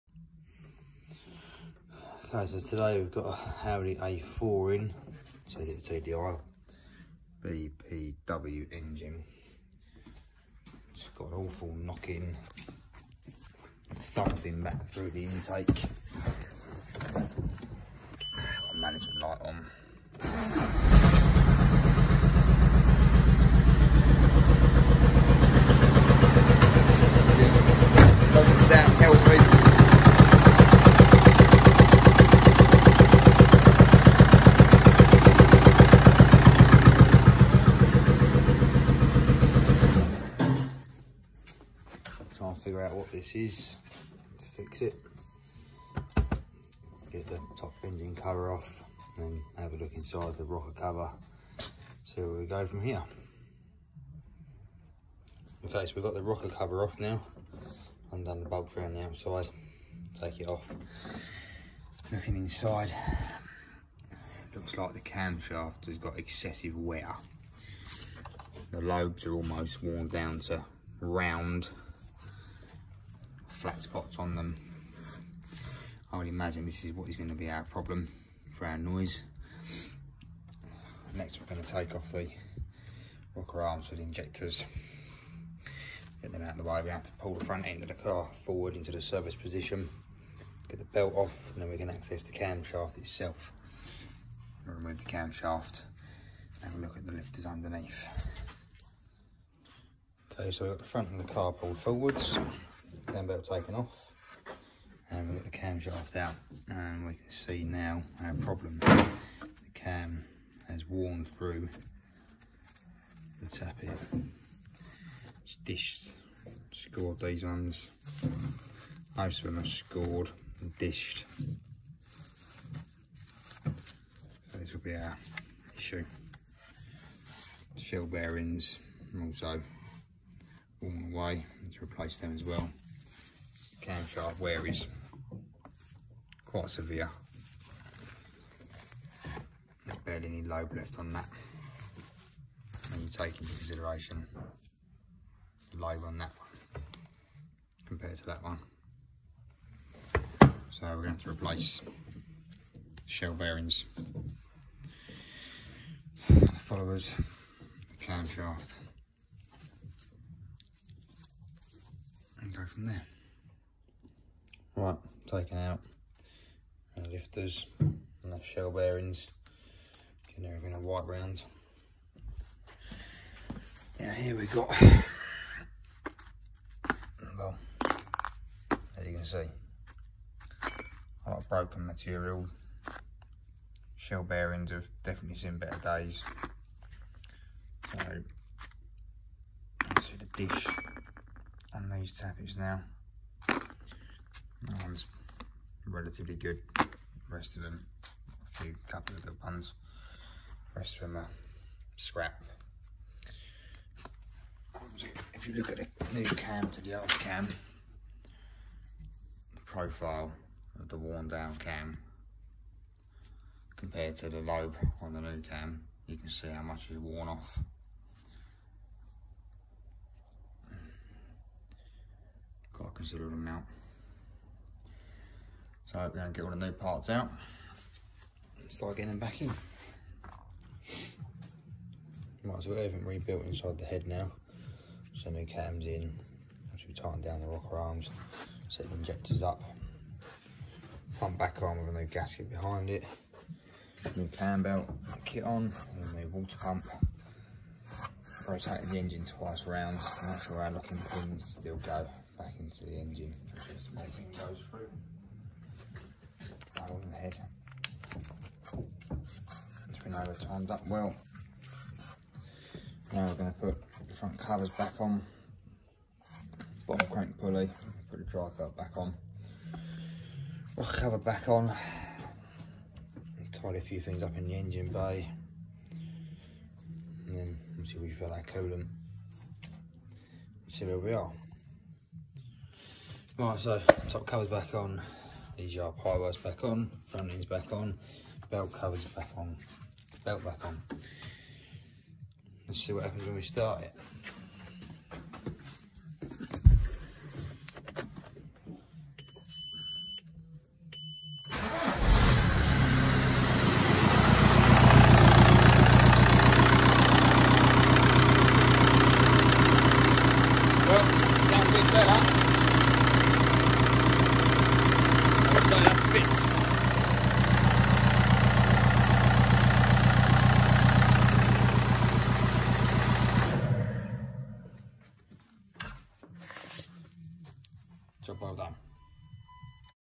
Audi /Vag 2.0 TDI (PD) Engine CAMSHAFT REPLACEMENT(worn camshaft and followers)
2.0 TDI camshaft and lifters replacement